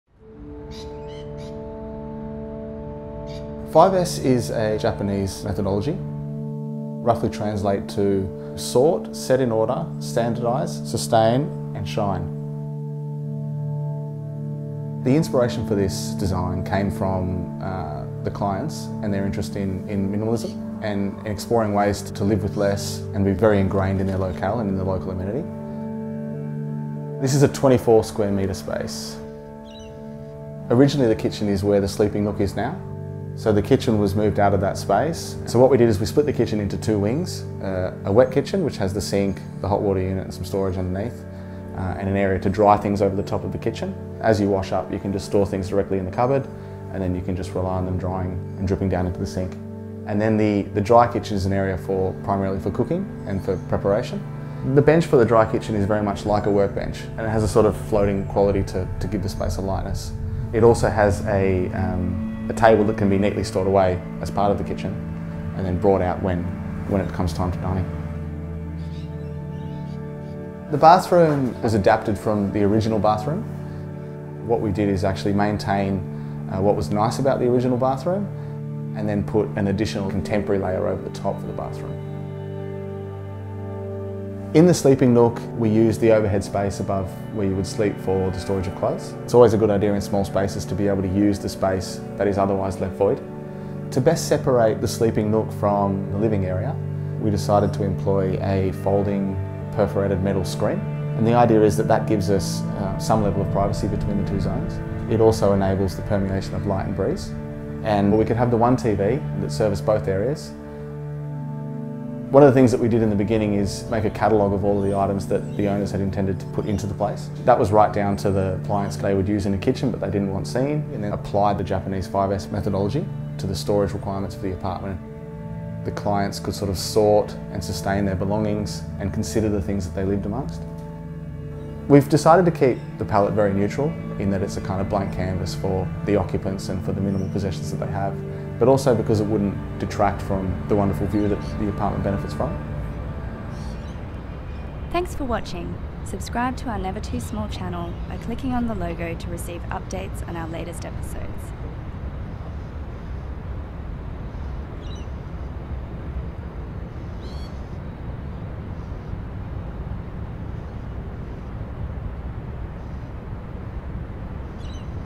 NEVER TOO SMALL Japanese 5S Micro Apartment - 24sqm/260sqft
Inspired by the Japanese 5S organisational method, architect Nicholas Gurney created a streamlined, minimalist 24m2 home. Neatly divided by a moving screens and a split wet and dry kitchen. Everything has a place in hidden storage including the portable cooktop and dining table, intended to never detract from the outdoor view.

Director: Colin Chee
Producer: Luke Clark
Camera: Colin Chee
Editor: Colin Chee